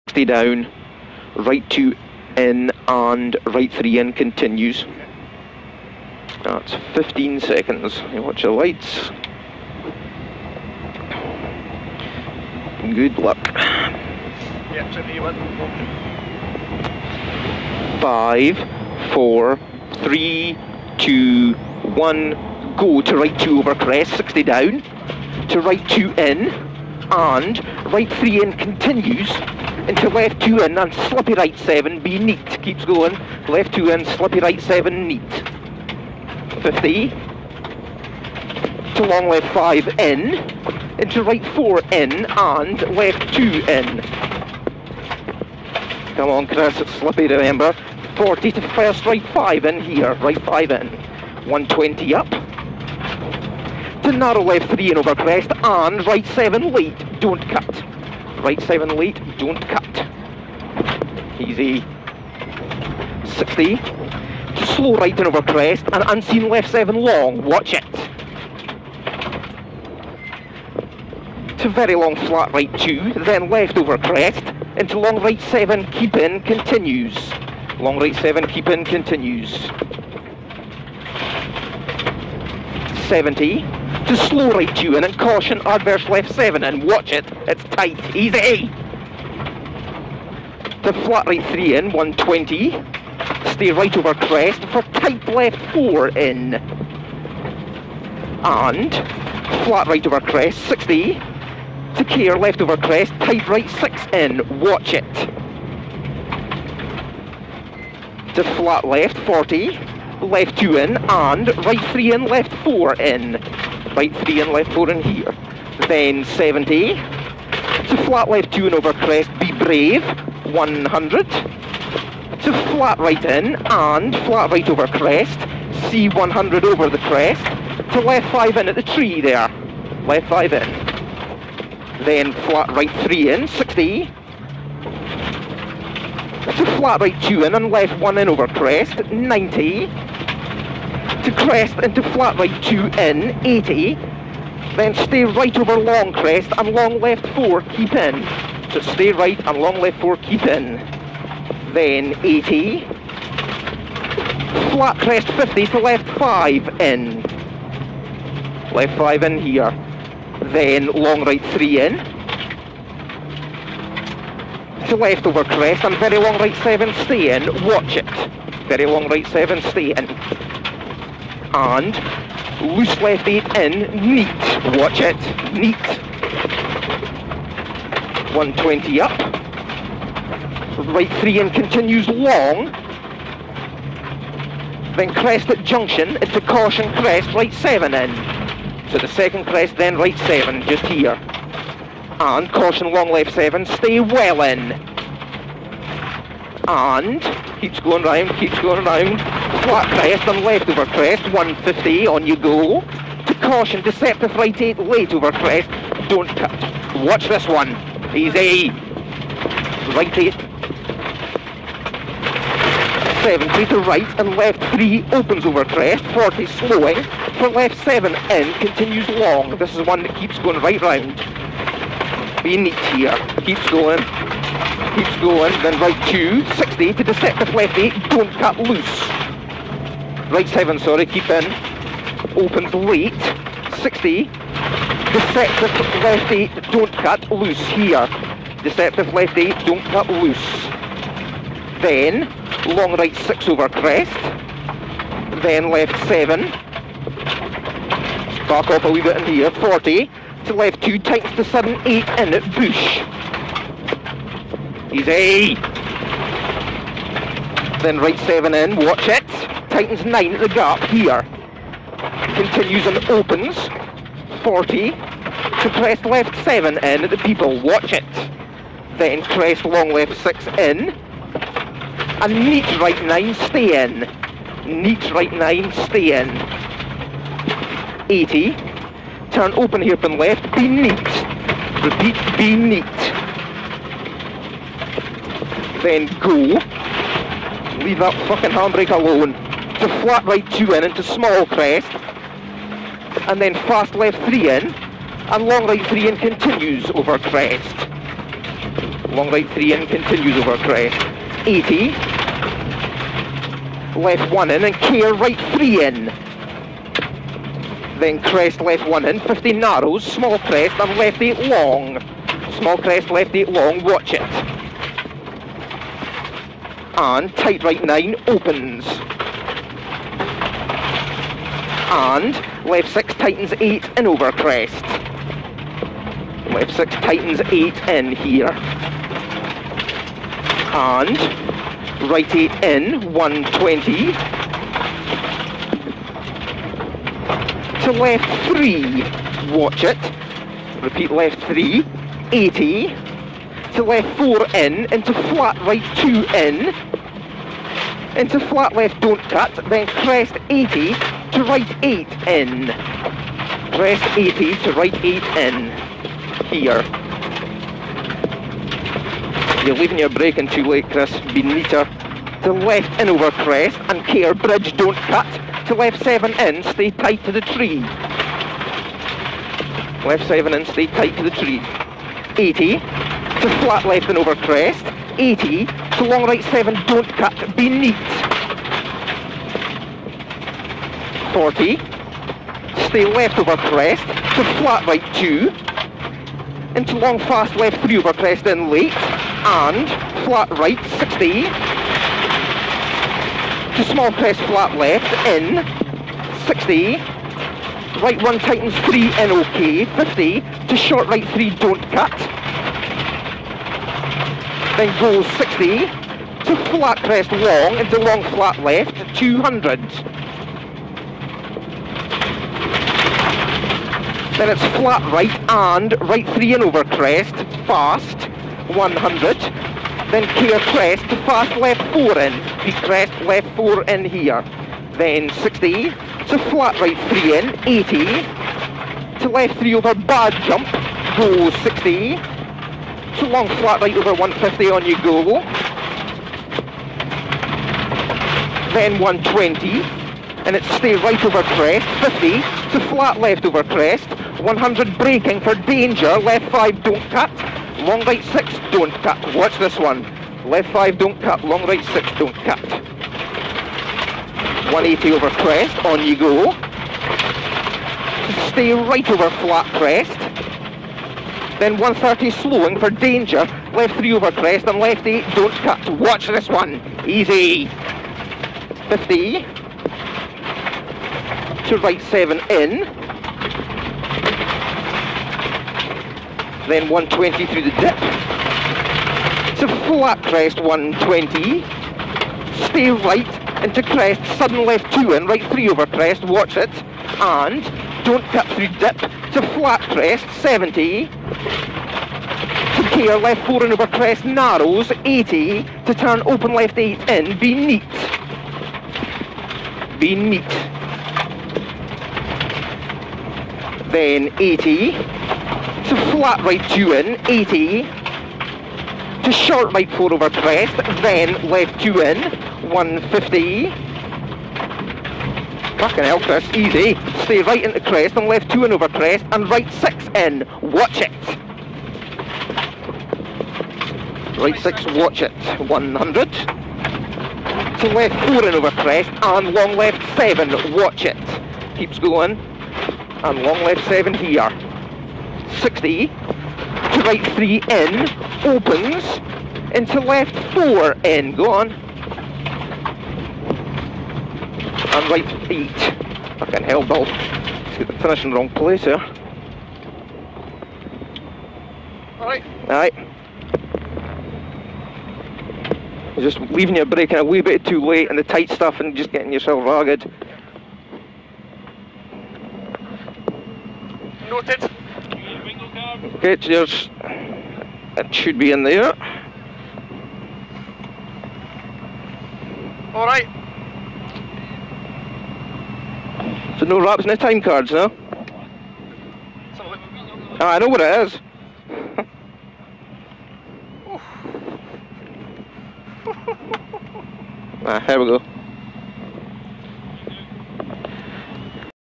Galloway - Stage 1 - Glengap 1
Galloway Hills Rally, 30 October 2011 - Chris Collie and Neil Shanks.
Stage 1, Glengap 1